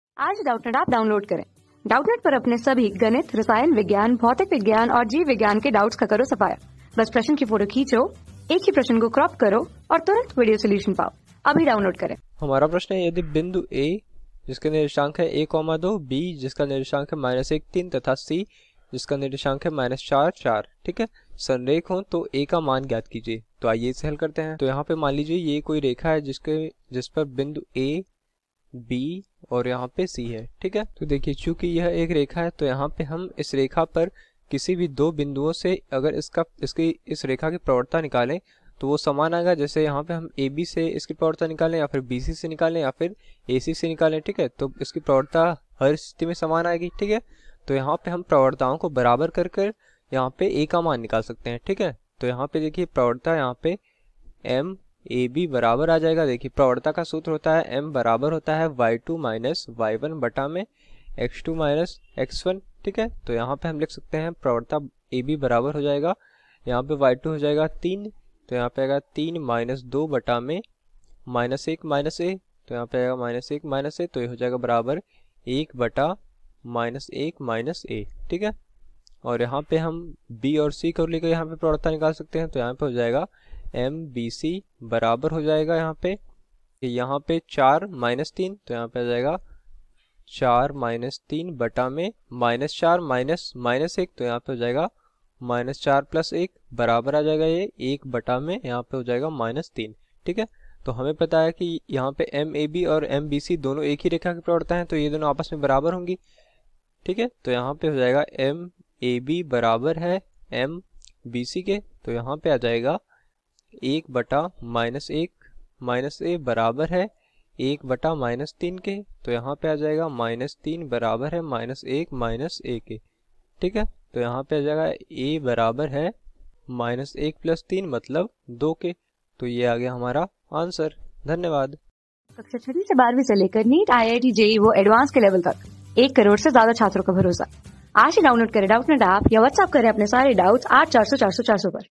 यदि A(a,2),B(-1,3)तथा C(-4,4)संरेख हो , तो का a मानज्ञात कीजिए । | 12 | सरल रेखा | MATHS | NAVB...
यदि A(a,2),B(-1,3)तथा C(-4,4)संरेख हो , तो का a मानज्ञात कीजिए ।
Class: 12
Subject: MATHS
Chapter: सरल रेखा
Board:Madhya Pradesh Board

👉 Have Any Query? Ask Us.